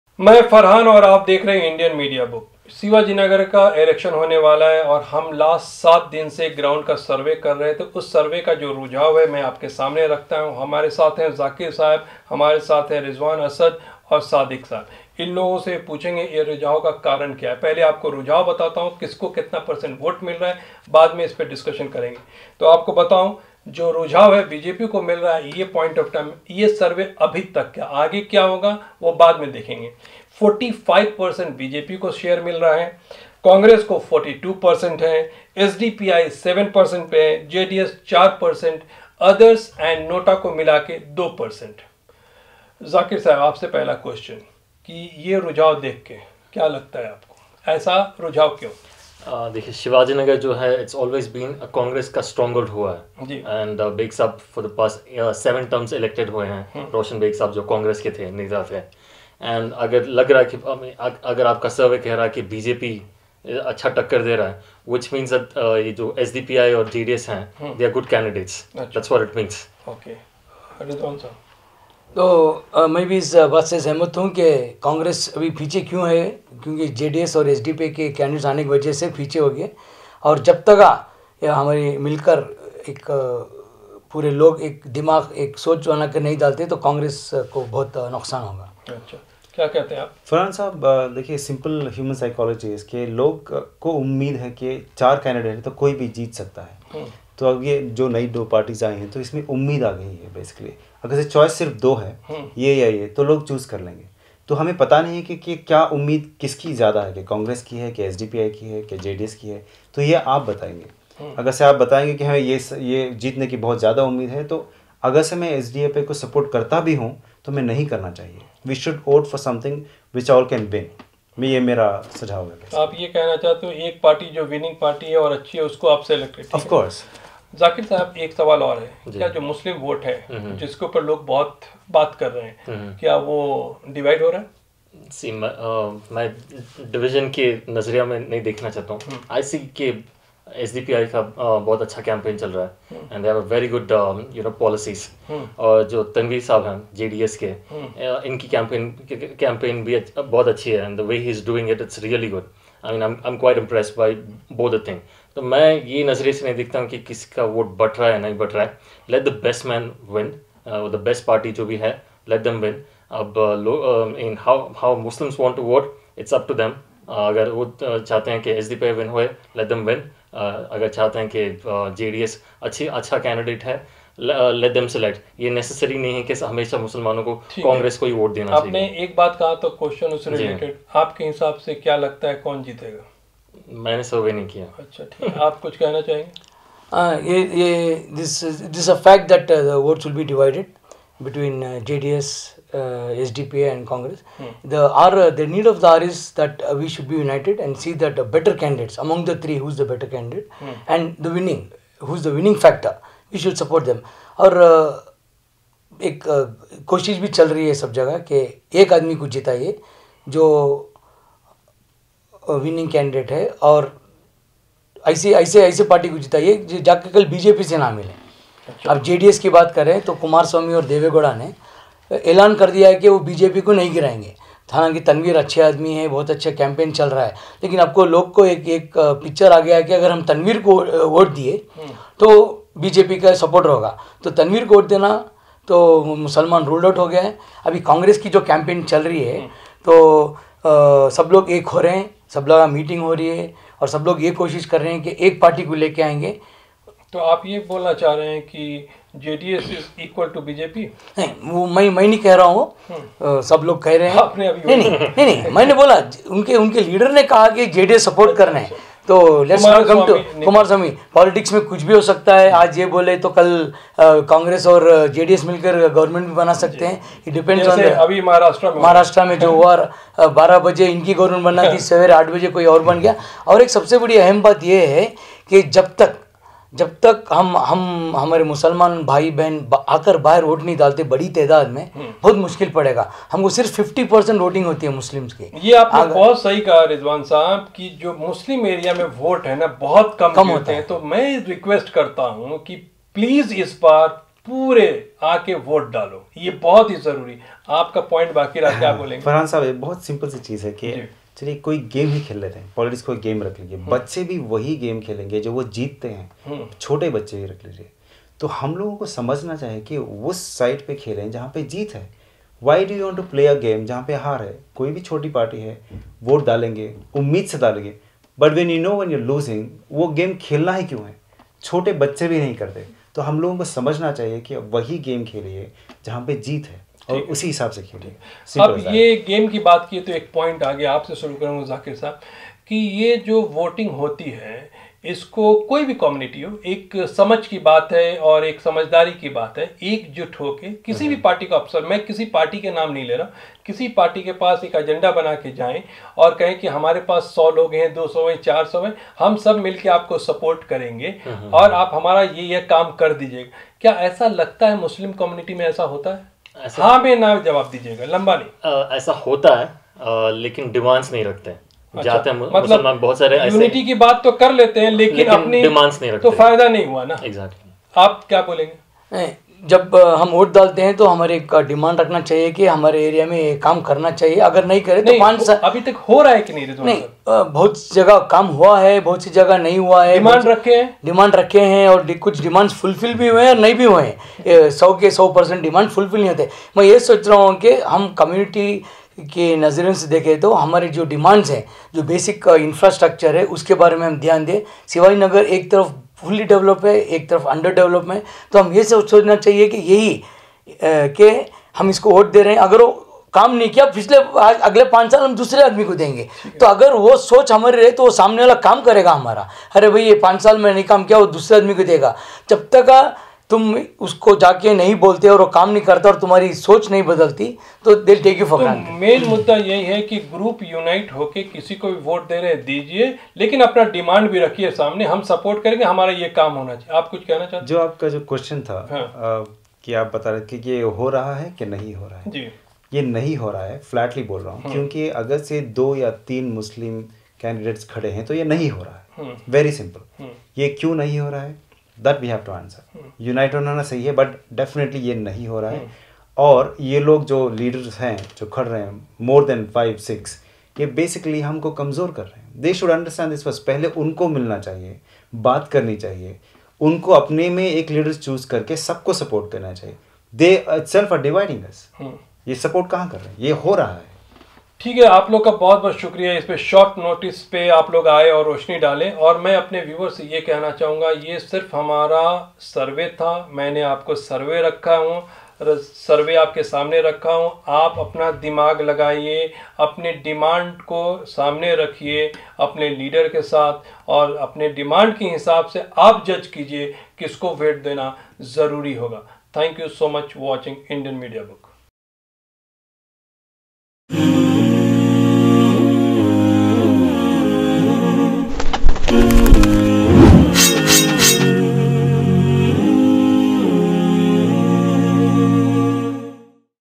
Shivajinagar_Assembly_Constituency BJP leads by more than 03 points. 23/11/2019.
Opinion polls so far show the BJP (Shivajinagar_Assembly_Constituency) in the lead, but with 11 days to go until election day there is still ample time for public opinion to shift.Voters will go the the polls on December 05.